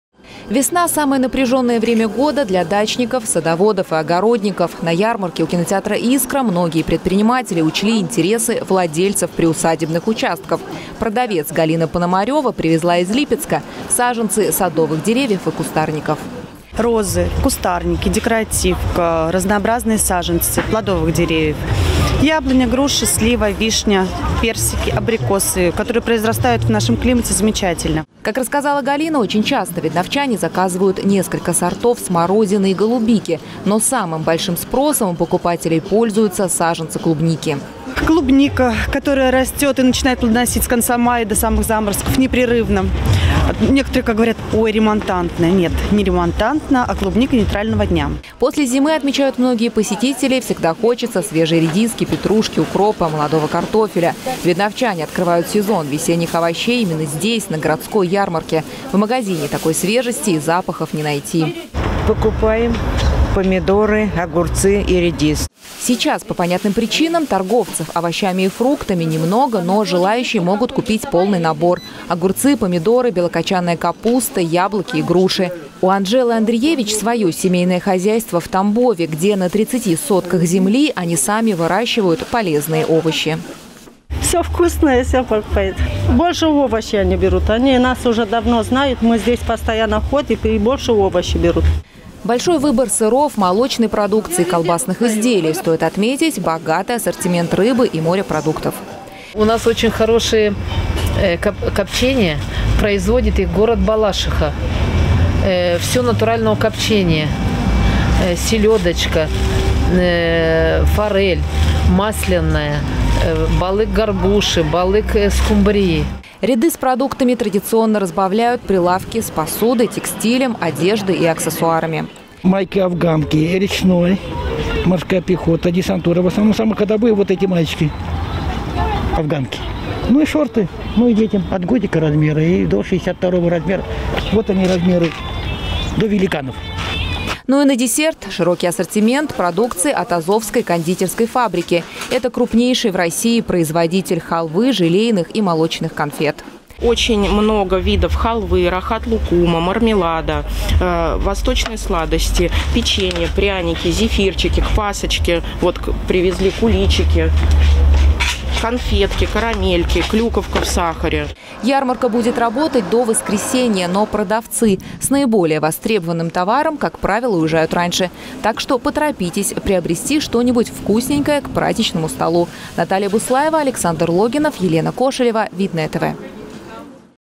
ГОРОДСКАЯ ЯРМАРКА
У кинотеатра «Искра» начала свою работу традиционная городская ярмарка. В широком ассортименте здесь представлена продукция крупных предприятий, фермерских хозяйств, личных подворий и индивидуальных предпринимателей из разных регионов нашей страны.